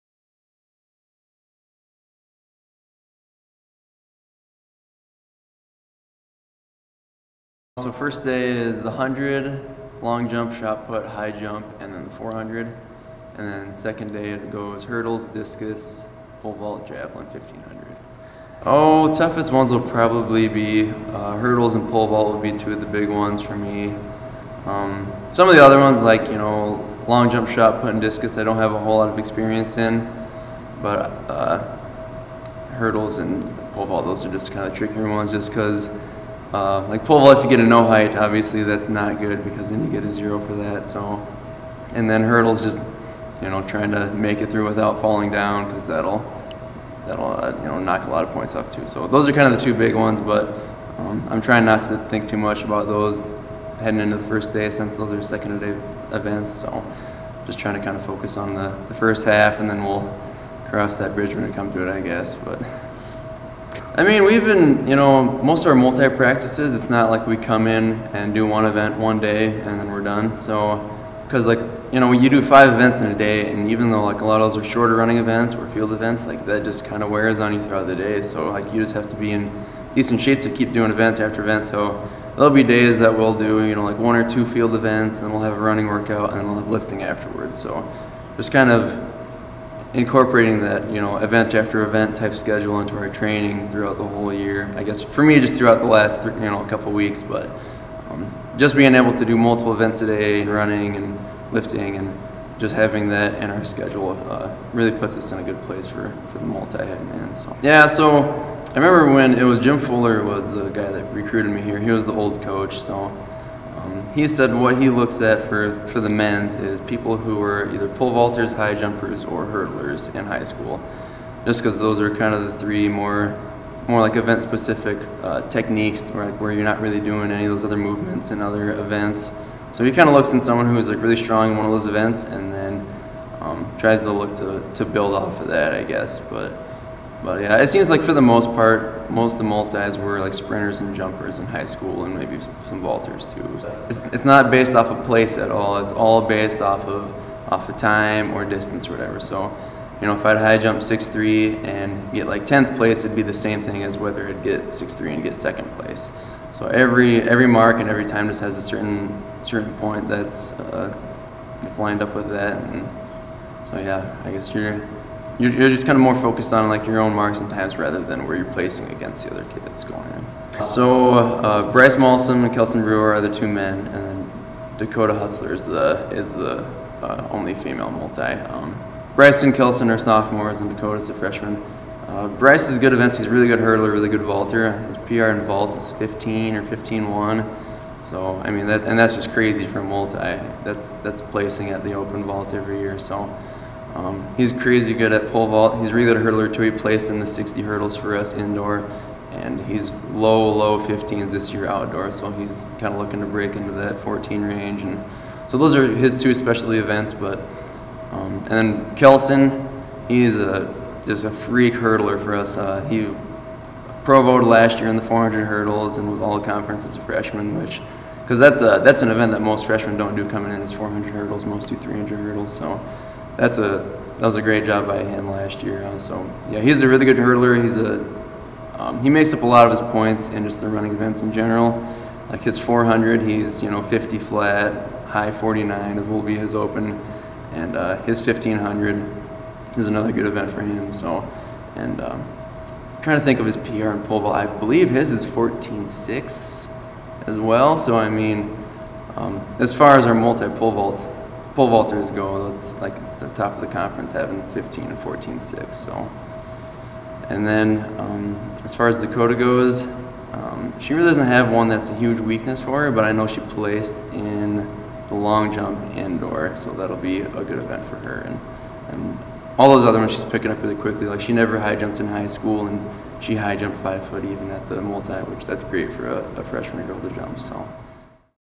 Multi event basics with Northern State's Monte Meyerink
Northern State track and field senior Monte Meyerink explains the decathlon and talks about his hardest events, what makes a good multi eventer and who his teammates are who compete alongside him.